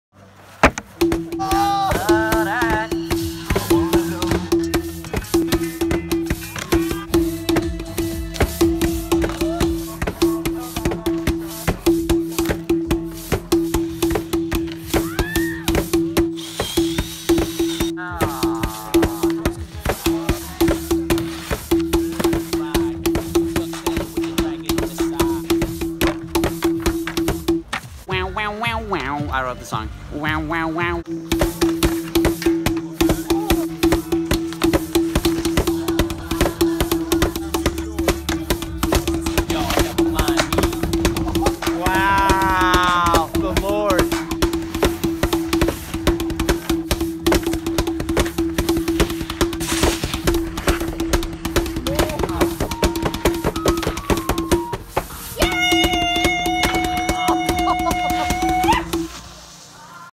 BMBW crew at Bench Heaven 2019
This is our Insta edit from "Bench Heaven" at Oslo Vinterpark. Great event. Thanks Fredrik Perry for putting it on! Go to bentmetal.com to see way more!
Video featuring Zebbe Landmark, Fredrik Perry and Brandon Reis. Phoned by Jesse Burtner, music by Gary Mandalls and the Gates